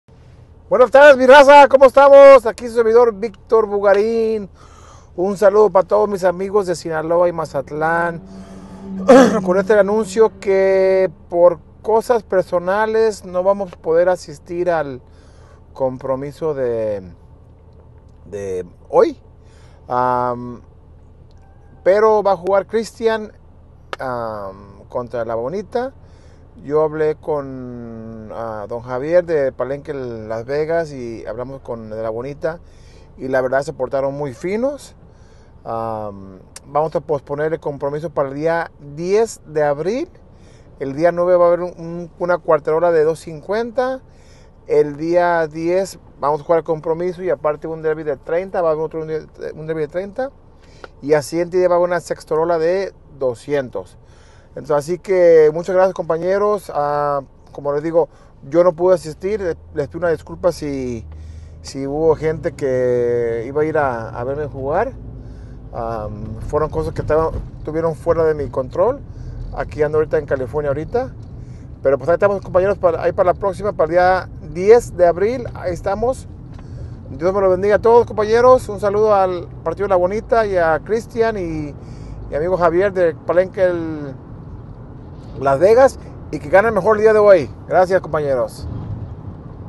Se pospone el compromiso
Se pospone compromiso de capotes de Durango para el día 10 de abril